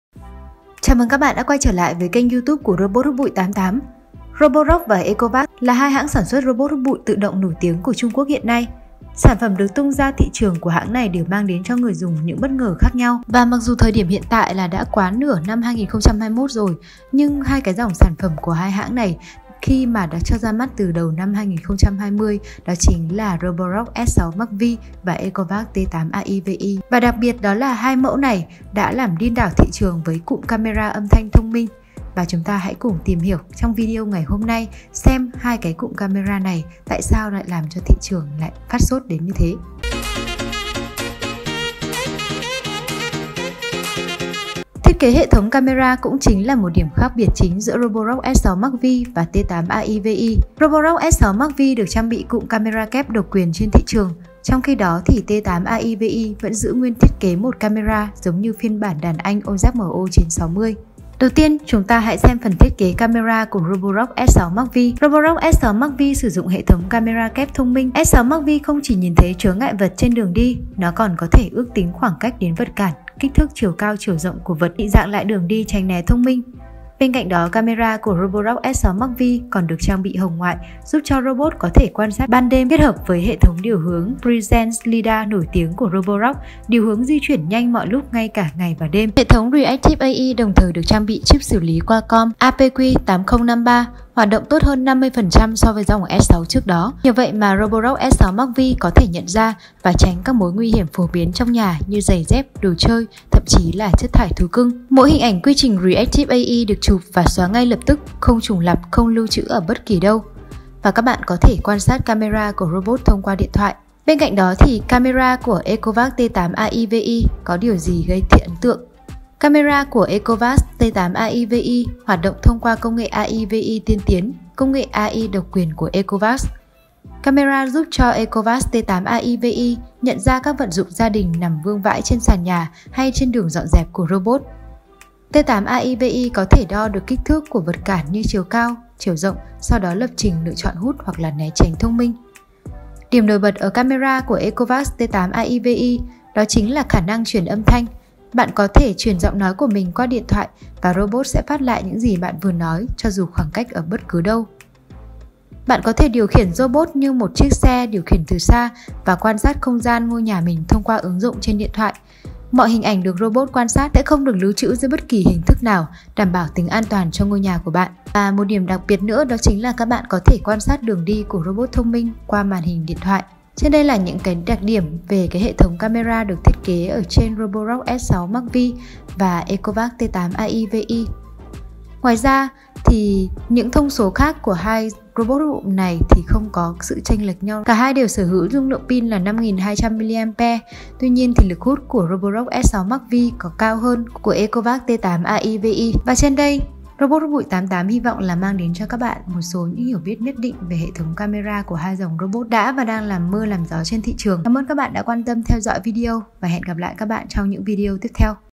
So sánh camera của robot hút bụi Roborock S6 MAXV và Ecovacs T8 AIVI
ROBOT 88 - ROBOT HÚT BỤI LAU NHÀ